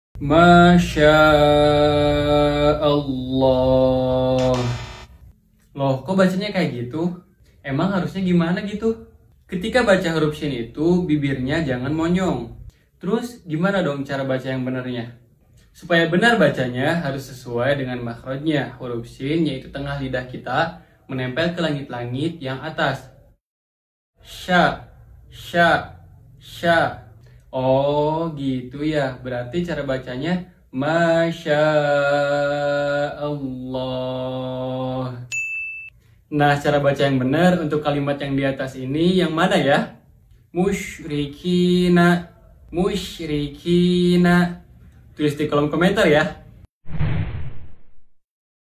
Baca huruf syin ش harus monyong?🤔belajarngaji
Baca huruf syin (ش) harus monyong? 🤔

📌 keliru jika membaca huruf syin monyong, karena tempat keluar huruf ini bukan dari kedua bibir tapi dari tengah lidah menempel ke tengah langit-langit atas, jadi bibir kiangak perlu monyong yah 👍🏻

Jika sobat Tartiilaa ingin bacaan makin fasih, langkah awal pelajari makhorijul huruf. Itu apa sih?
Makhrijul huruf ➡️ tempat-tempat keluar huruf.. kuasai ini InsyaAllah bacaan akan lebih baik dan sering praktekin dengan baca Quran 🥰

Follow instagram kita yah @TARTIILAAMENGAJI
🔖 Share juga yah biar yang lain dapet manfaatnya dan kita dapet amal jariyyah ✨